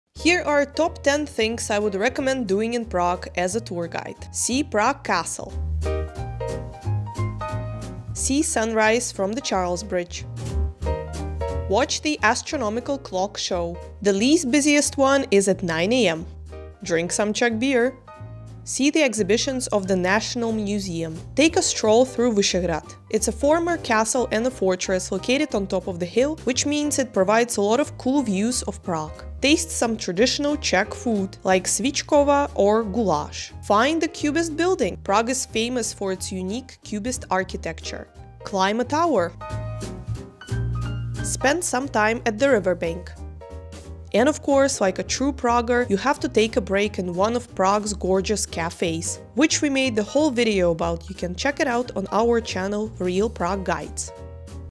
TOP 10 Things to Do in Prague in 2024
These are the TOP 10 Things do in Prague in 2024 from Prague's Guide perspective.